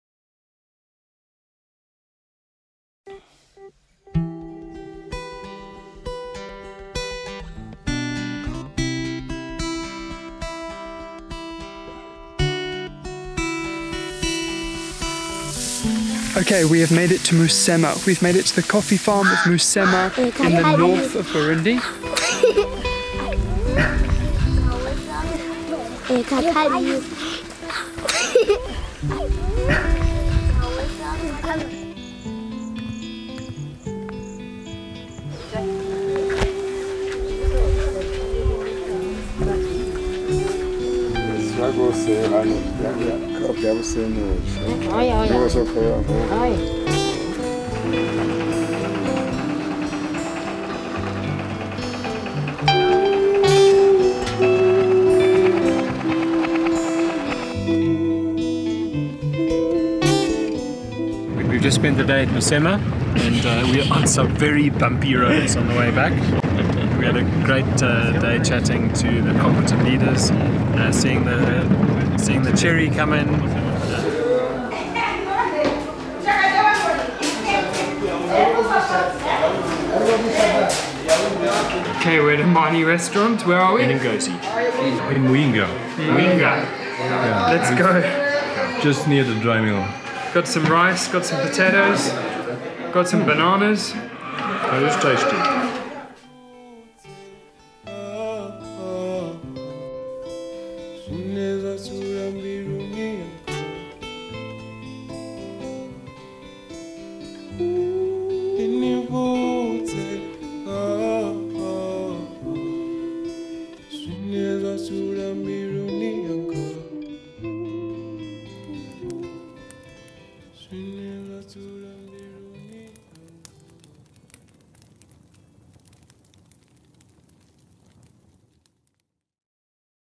Bean There Coffee in Burundi
The Musema Cooperative is located in the Kayanza province of northern Burundi not far from the town of Ngozi.

Bean There began purchasing coffee from Musema in 2011 and we have seen the demand for this exceptional coffee increase every year since. The cooperative is small but committed to excellence which can be seen in the agronomy training made available to farmers, and the improvements in processing techniques at the washing station, including using Ecopulpers which use less water than conventional pulpers.

We have visited Musema regularly since 2011 and we are encouraged to see the development and growth of the cooperative which has resulted in increased yields, improved quality and increased income to farmers. We currently purchase all the coffee that Musema can produce and although the cooperative has no official certification Bean There is committed to paying a Direct Fair Trade premium to the farmers.